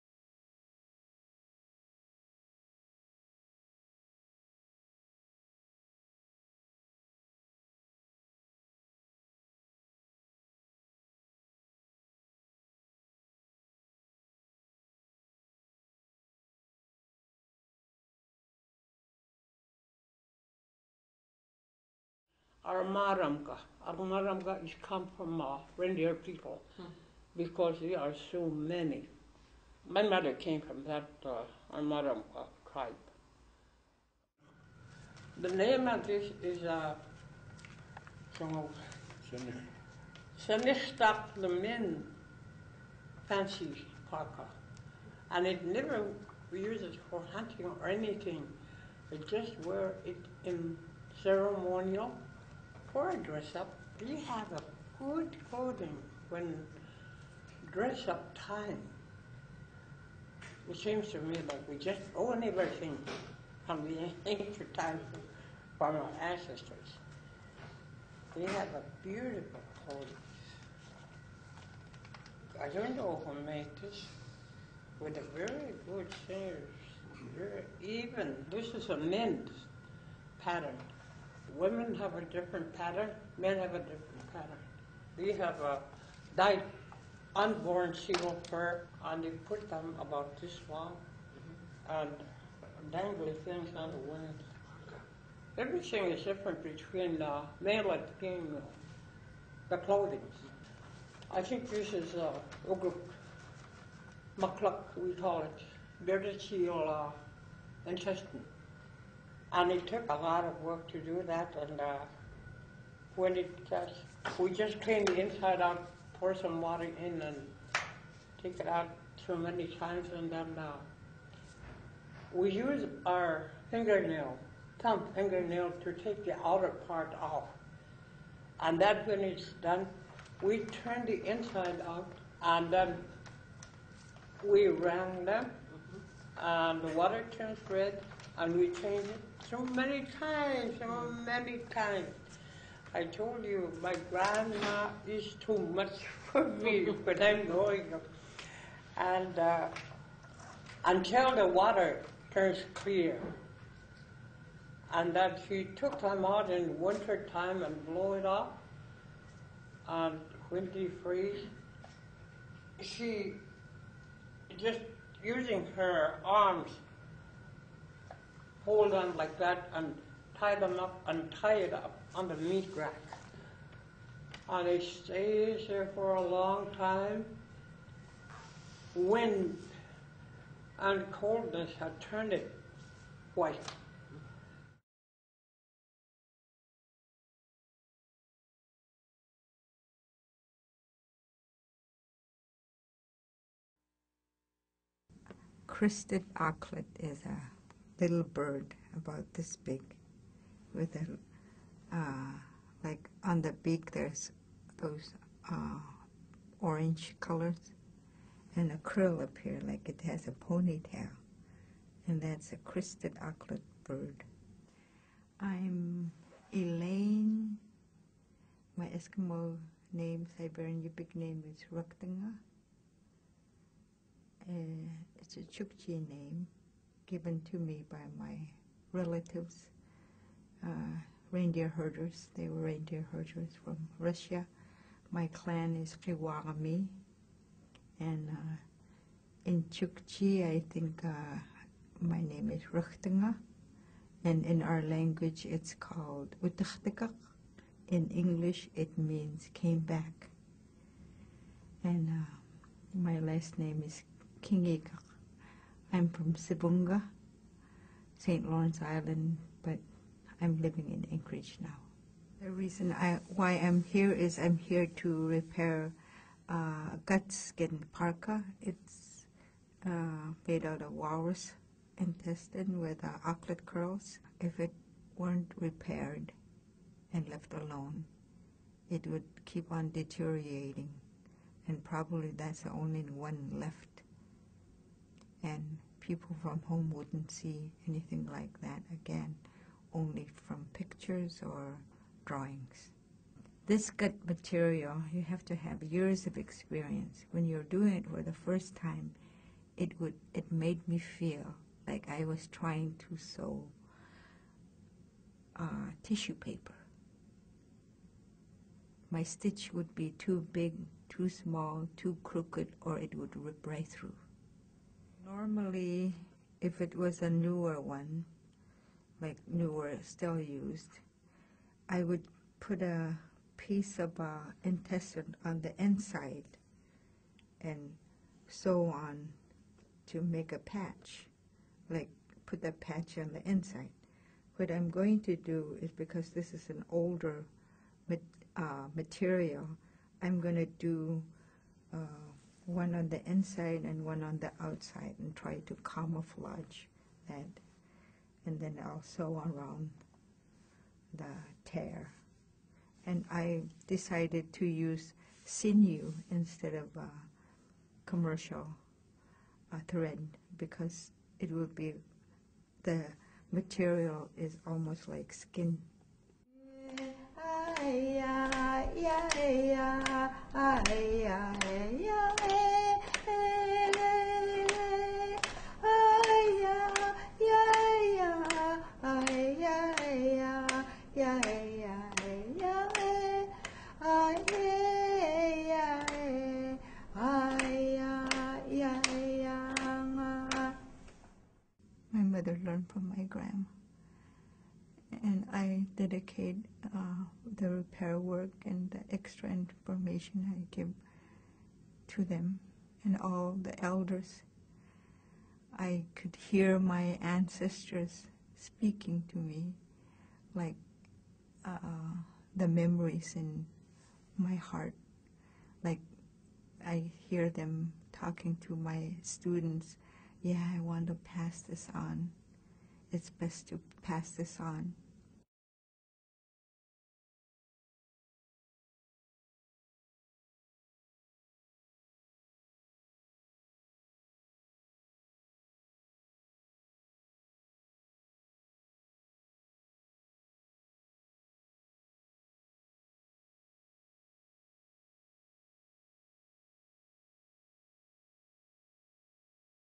Gut Parka Informational Video
Two different experts shed light on the gutskin parka-- how they were traditionally made, and how they are conserved today.

This video is part of the "Tools" exhibition at Cooper Hewitt, Smithsonian Design Museum from December 2014 to May 2015.